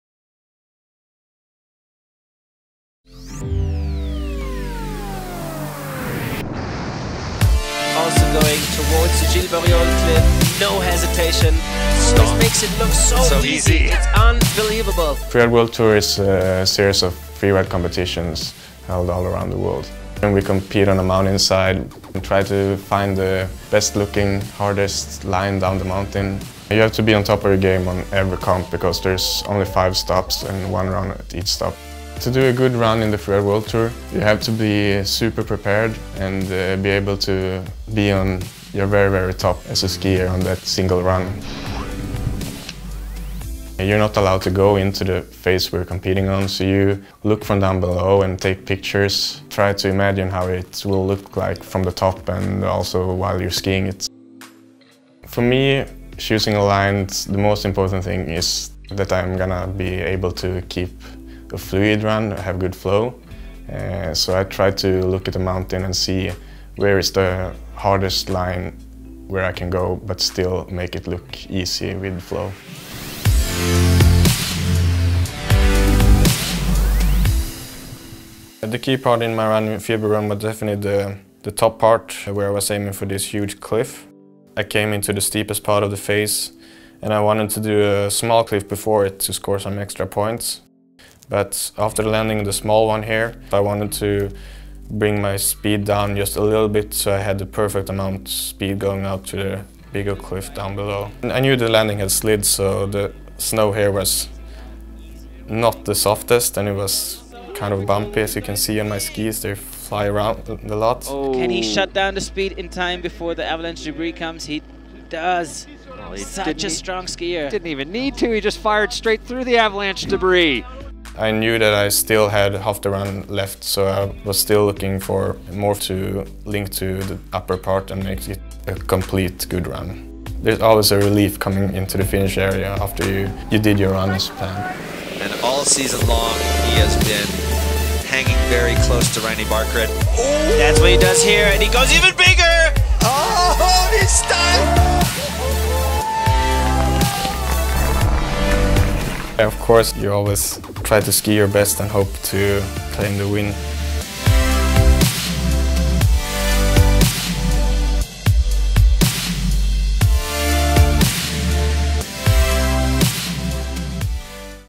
Behind a Radical Line
Kristofer Turdell, the world’s #3 ranked freeskier, explains the complexities of skiing a Freeride World Tour venue.